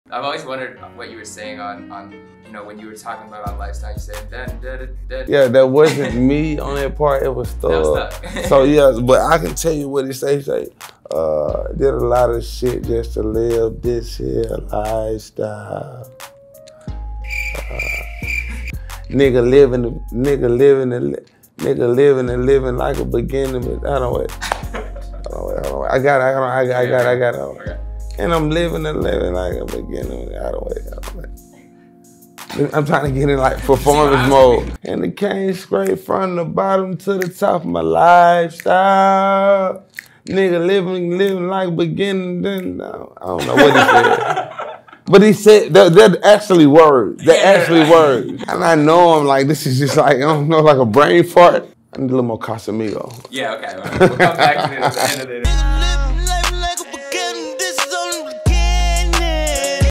Rich Homie Quan Had NO CLUE What Young Thug Was Singing 😂
Rich Homie and Young Thug are a dynamic duo